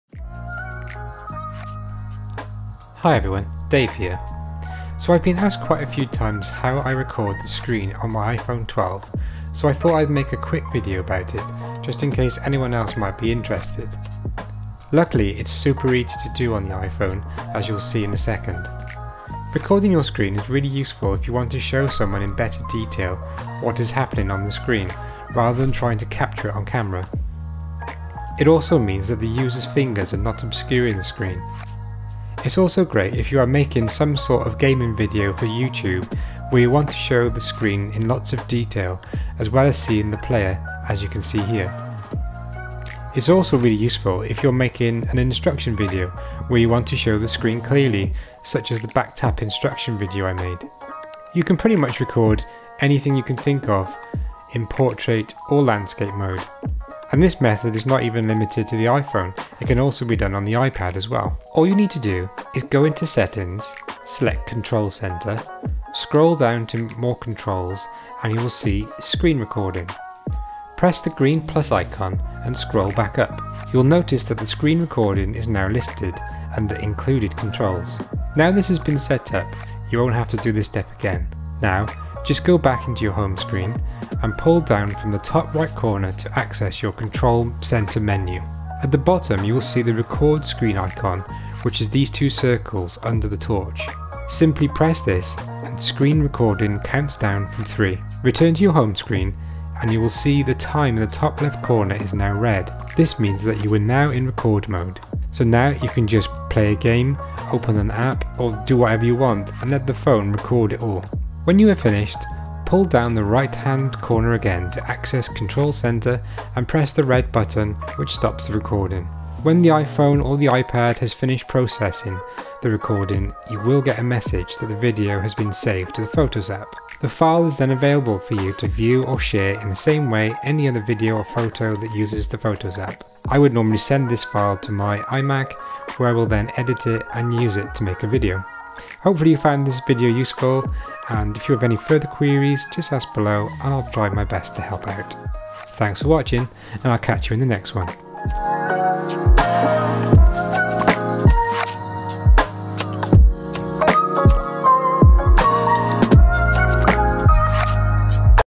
How to SCREEN RECORD on iPhone
In this video I show you how to screen record iPhone 12.  This works on iPhone 14 pro as well.

So i’ve been asked quite a few times how I record the screen on my iPhone 12 so I thought i’d make a quick video about it just in case anyone else might be interested.  Luckily it’s super easy to do on the iPhone as you’ll see in a second.  Recording your screen is really useful if you want to show someone in better detail what is happening on the screen, rather than trying to capture it on camera.  It also means that the users fingers are not obscuring the screen.  It’s also great if you are making some sort of gaming video for youtube where you want to show the screen in lots of detail as well as seeing the player as you can see here.  It’s also really useful if you’re making an instruction video where you want to show the screen clearly such as the back tap instruction video I made.  You can pretty much record anything you can think of in portrait or landscape mode.  This method is not limited to the iPhone either, it can also be done on iPad’s as well. It's a great ios screen recording method.

So, all you need to do is to go into settings, select control centre scroll down to more controls and you will see Screen Recording.  Press the green plus icon and scroll back up.  You will notice that screen recording is now listed under included controls.  Now this has been set up, you won’t have to do this step again. Now just go back to your home screen, and pull down from the top right corner to access your control centre menu.  At the bottom you will see the record screen icon which is these 2 circles under the torch.  Simply press this and screen recording counts down from 3.  Return to your home screen and you will see the time in the top left corner is now red.  This means that you are now in record mode. So now you can just play a game, open an app or do whatever you want and let the phone record it all.  When you are finished, pull down the right hand corner again to access control centre and press the red record button which stops the recording.  When iphone or ipad has finished processing the recording you wll get a message that the video has been saved to the photos app.

The file is then available for you to view or share in the same way any other video of photo that uses the photos app.  I would normally send this file to my iMac where I would then edit it and use it to make a video.

Hopefully you found this video useful and if you have any further queries just ask below and i’ll try my best to help out.